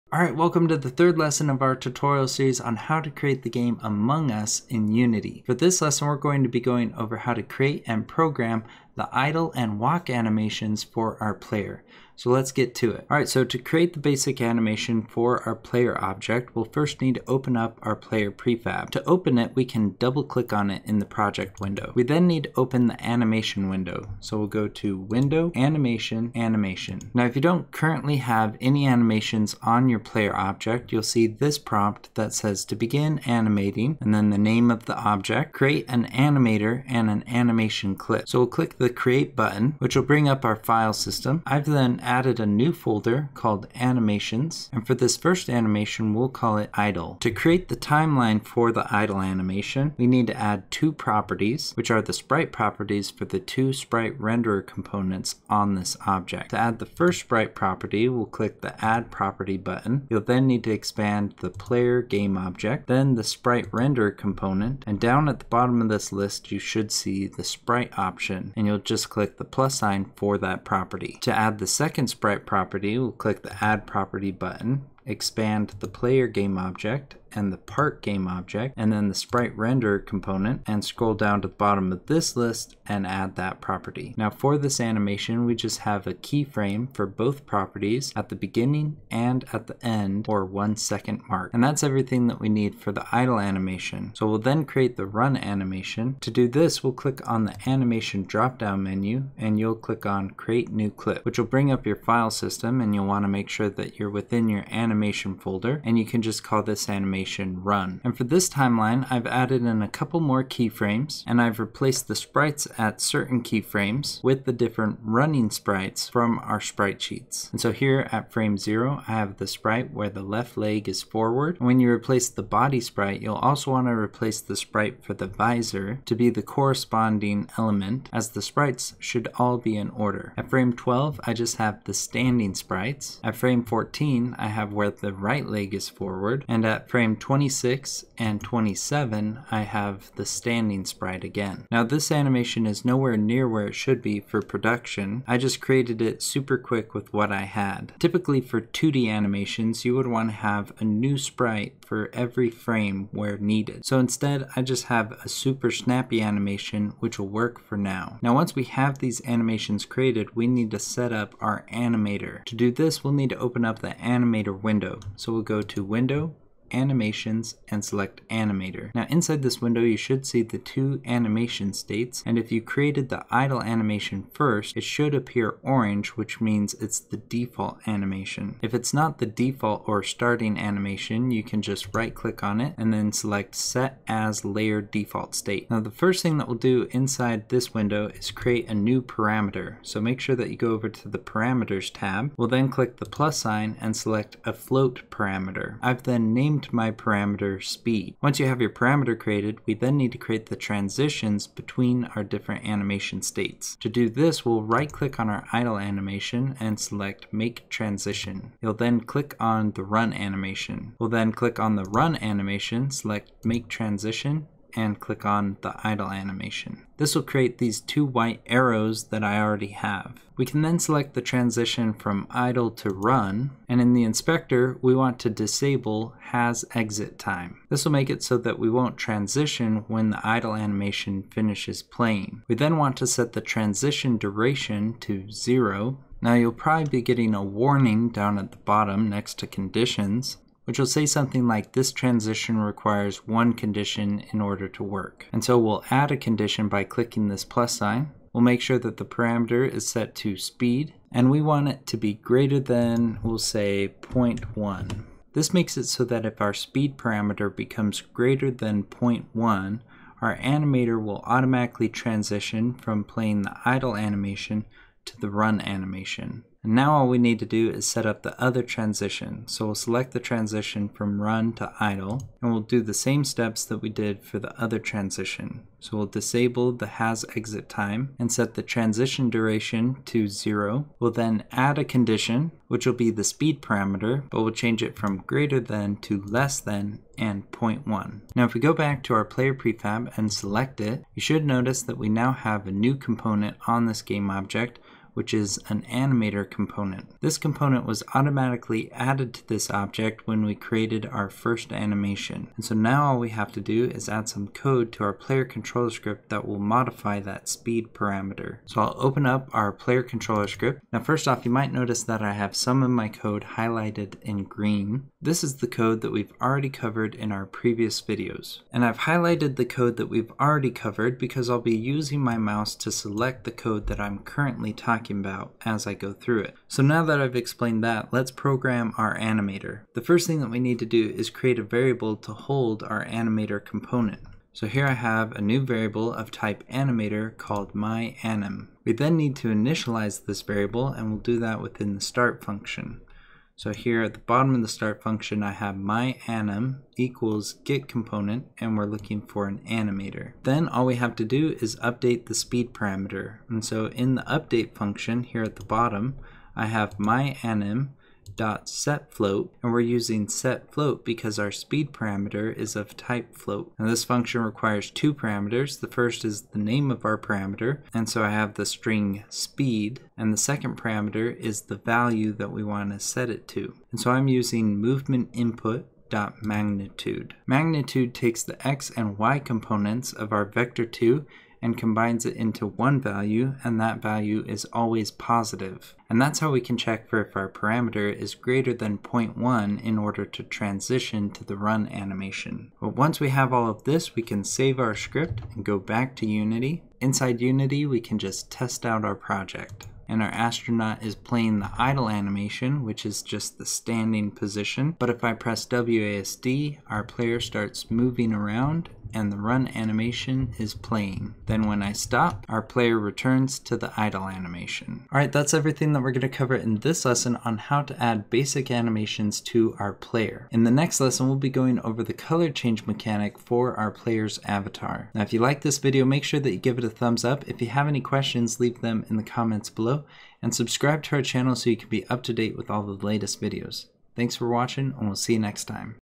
Among Us in Unity - Movement Animator (Lesson 3)
In this lesson on how to create Among Us in Unity, we will be creating animations and setting up our player's animator. The first thing we need to do is create our animations and for now, we only need an idle animation and a run animation. Once we have created the animations we then need to set up the animator for our player and then transitions between animations. Finally, we will need to go into our player controller script and add some code that will modify the parameters of our animator so the animations will transition automatically.